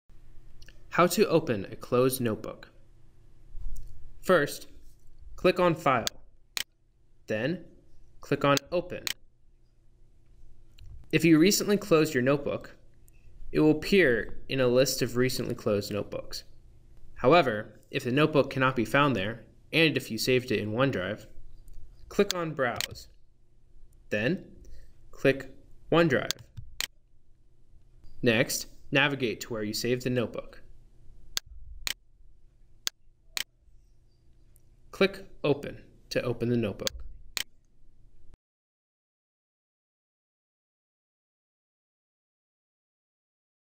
Office365 - (ONENOTE) Opening a Closed Notebook
How to Open a Closed Notebook in OneNote 2016

Recorded by Jon Prentice

Produced by the SMCHS IT Department Interns for use by Santa Margarita Catholic High School